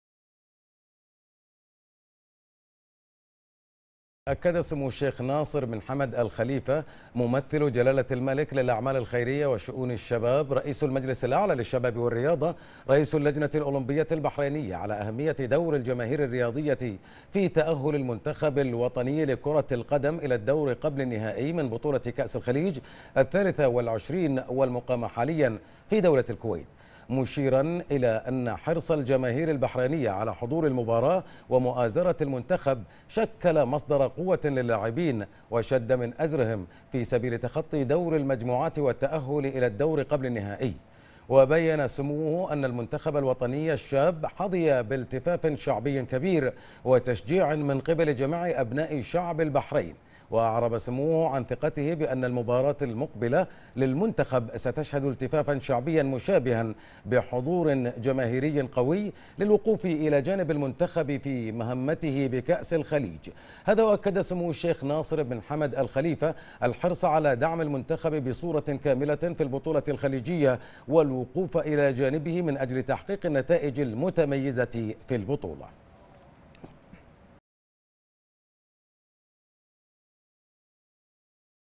البحرين : ناصر بن حمد: المنتخب الوطني حظي بالتفاف شعبي كبير والجماهير عودتنا على دعم الاحمر
أكد أنها شكلت مصدر قوة للمنتخب ...ناصر بن حمد: المنتخب الوطني حظي بالتفاف شعبي كبير والجماهير عودتنا على دعم الاحمر